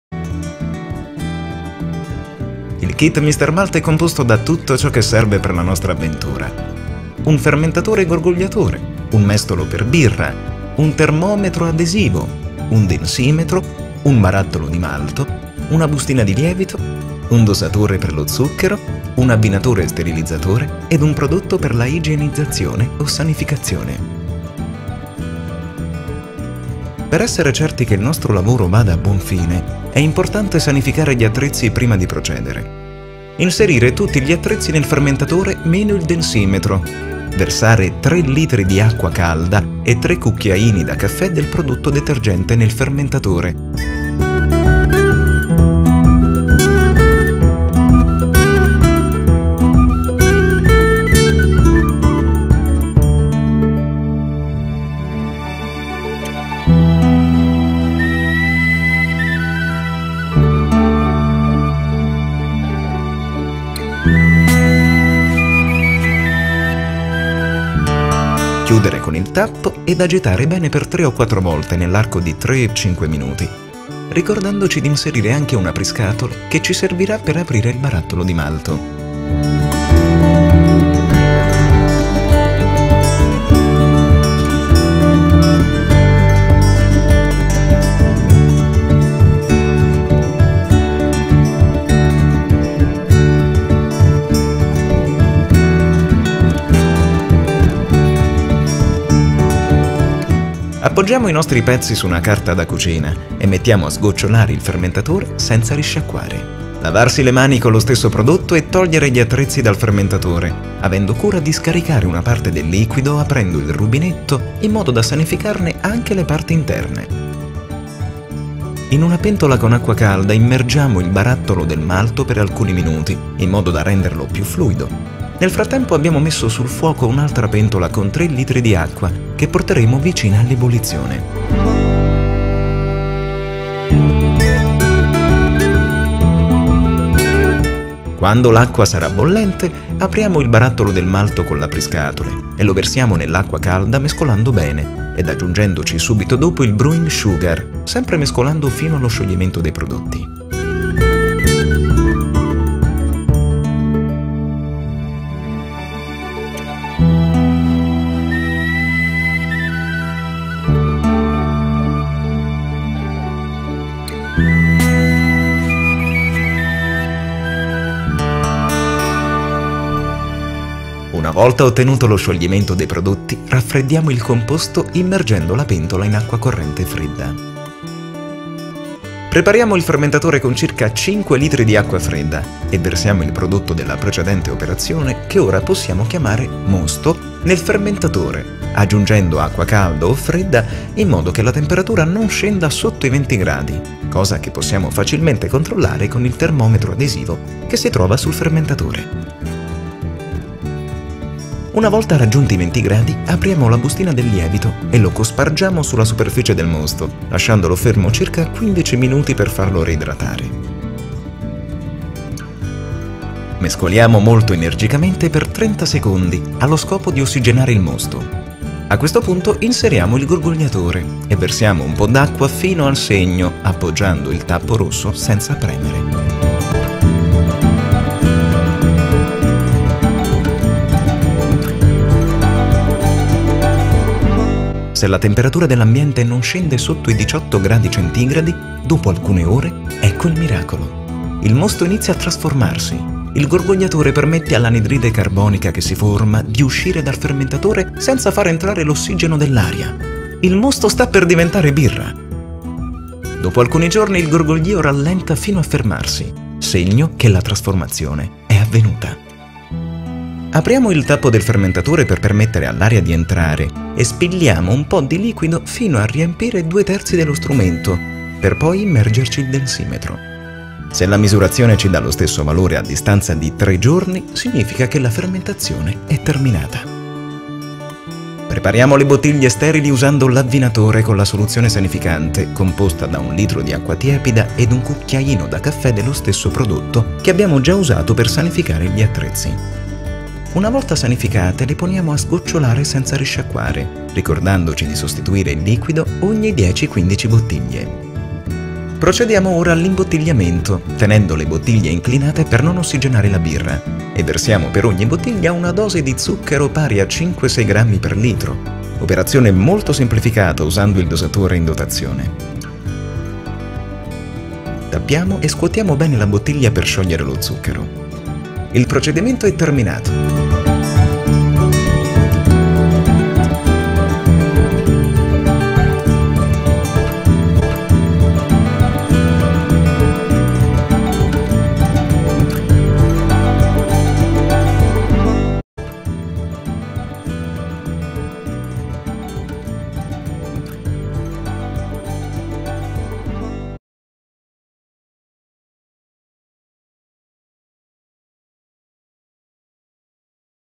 Home Brewing in Malta
How to make your own beer with the home brewing starter kits.
Brewing beer at home is fun, simple, convenient and cheap! End result is  a personalised and great quality home made beer.

Free Delivery in Malta!

online shop for Home Brewing and Craft Beer